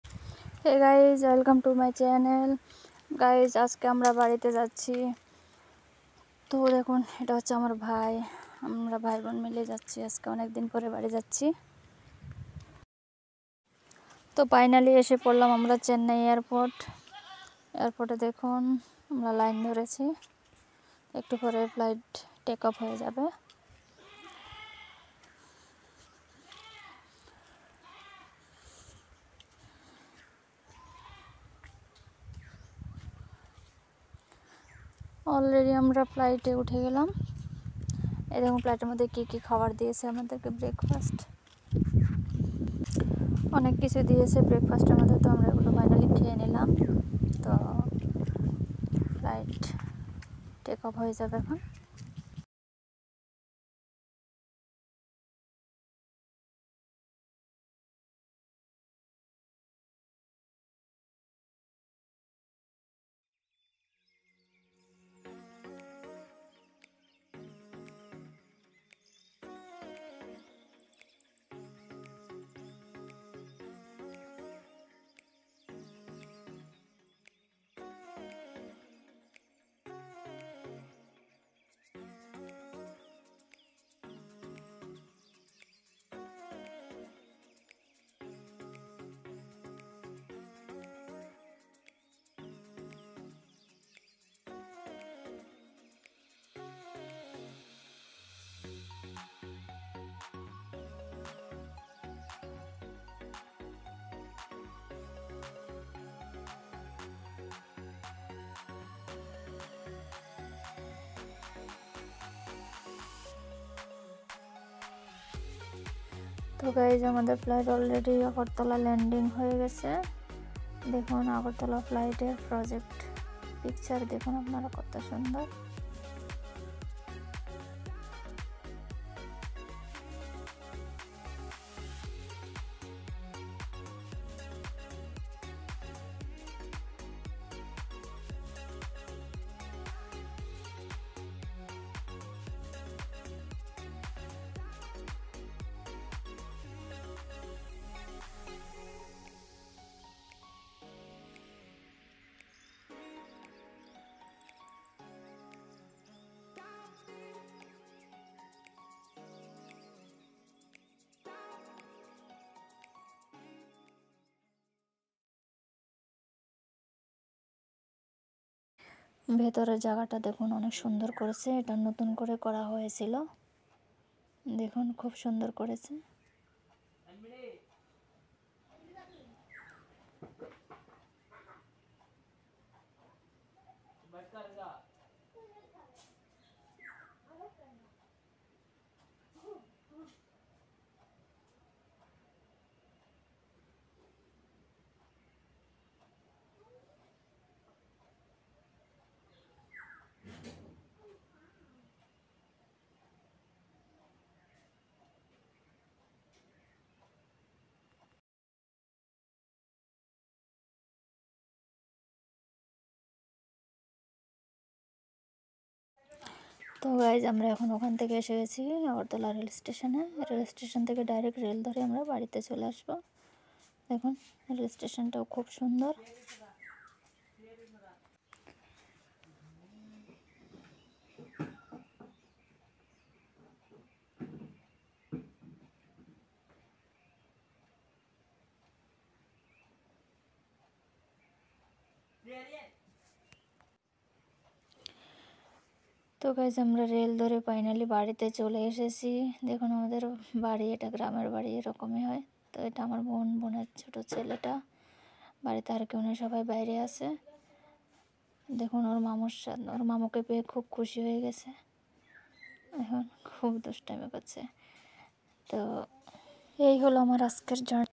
Chennai to Agartala...✈️✈️ Flight Journey Blog///চেন্নাই থেকে আগরতলা ,,,❤️
chennai to Agartala Flight Journy